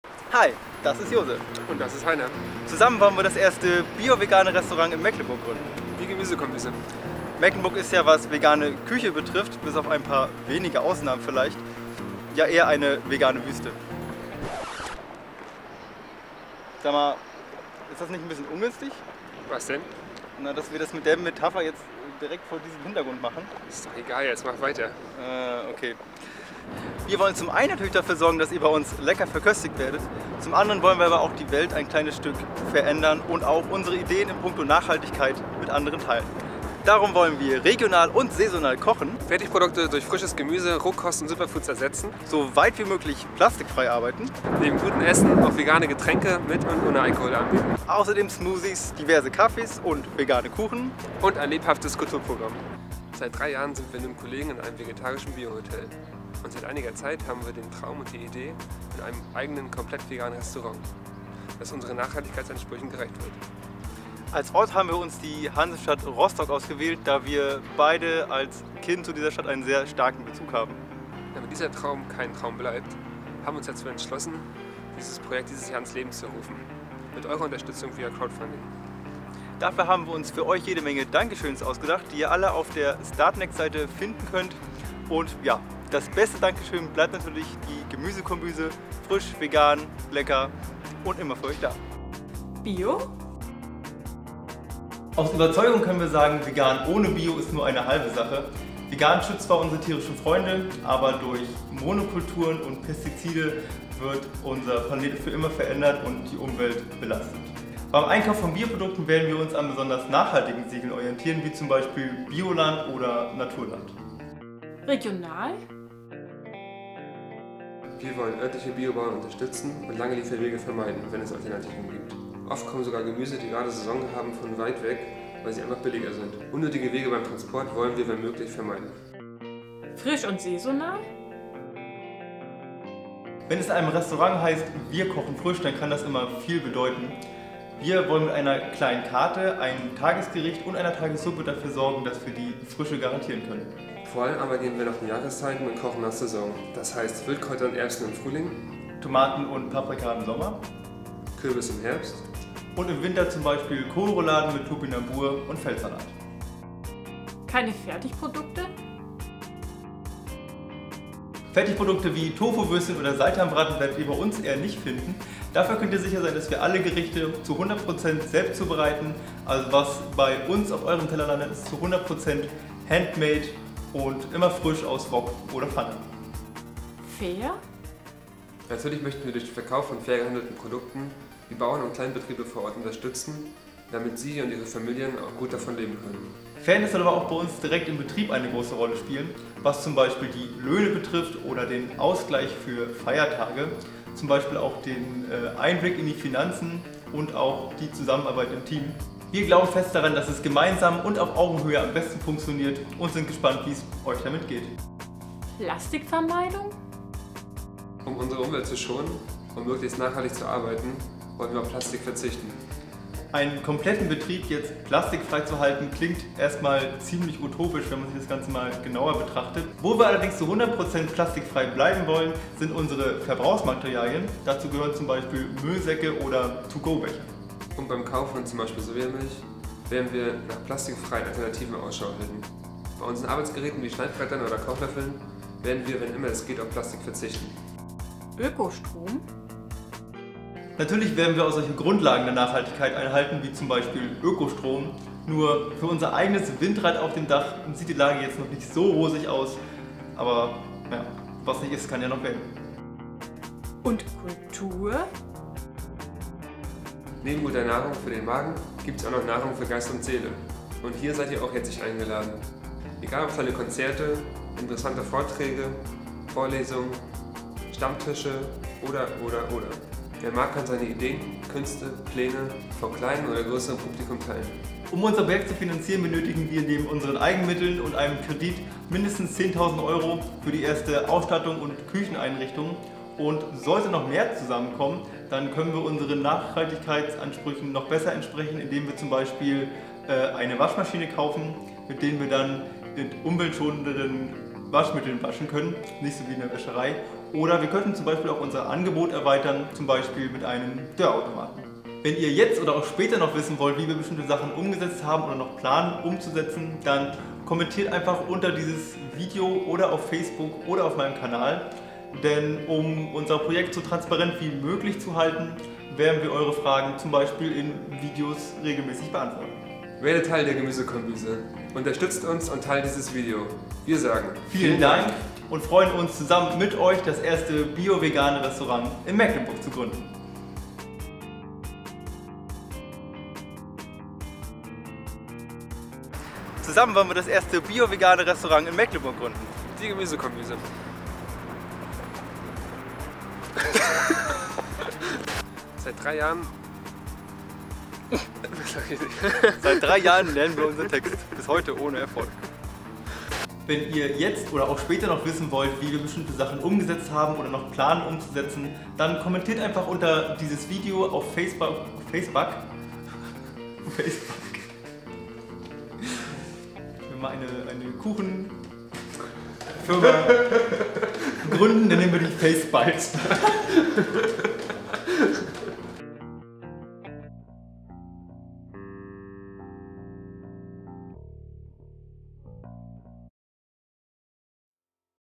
Grüne Kombüse - Crowdfunding-Kampagne für ein bio-veganes Restaurant
Wir sind Heiner und Josef und wir wollen das erste bio-vegane Restaurant in Mecklenburg gründen. Als Standort haben wir uns die Hansestadt Rostock ausgesucht.
Unser Speisenangebot soll sich über vegane Klassiker erstrecken, sowie über Rohkostgerichte. Die Gerichte werden saisonal ausgerichtet sein, ausserdem werden wir wenn möglich unsere Ware von regionalen Produzenten beziehen. Unsere Karte soll klein und flexibel bleiben, damit wir das bewerkstelligen können, dazu wird es ein Tagesgericht und eine Tagessuppe geben.
Zu trinken wird es vegane Kaffeespezialitäten, Smoothies, Limos, vegane Weine und Biere geben.
Wir möchten so gut es geht Plastikfrei bleiben. Wie genau wir das umsetzen werden, werdet ihr regelmäßig auf diesem Kanal erfahren.
Unterstützt uns bei unserem Vorhaben und bekommt tolle Dankeschöns dafür. Teilt dieses Video und verbreitet unsere Idee damit möglichst viele Menschen von unserem Projekt erfahren.
Danke :)